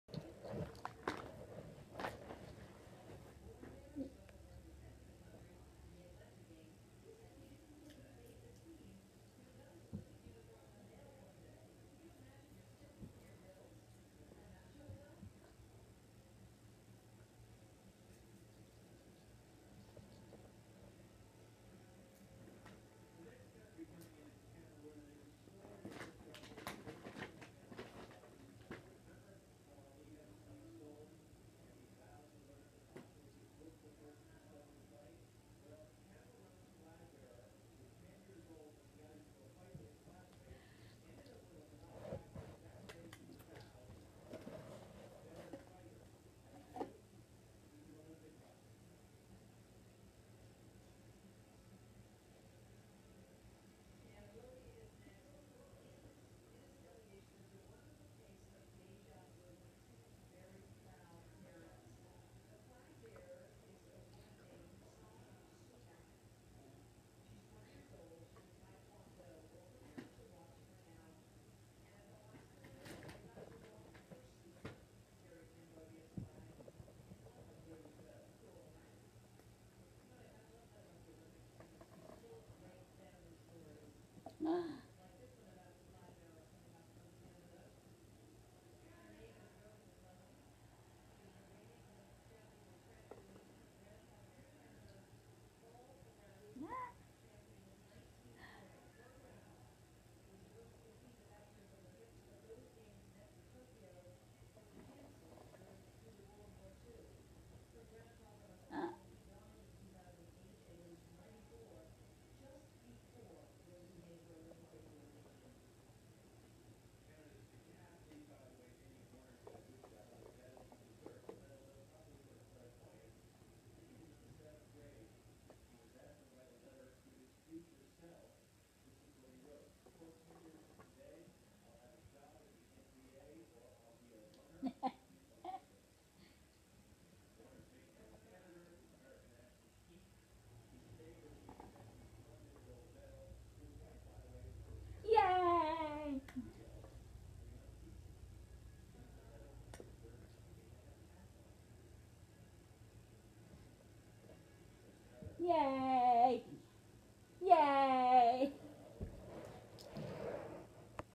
Buster is introduced to a new trick....
Buster was introduced to a new trick today...tracking a hidden object. His first time trying to find an object that is hidden, and moves.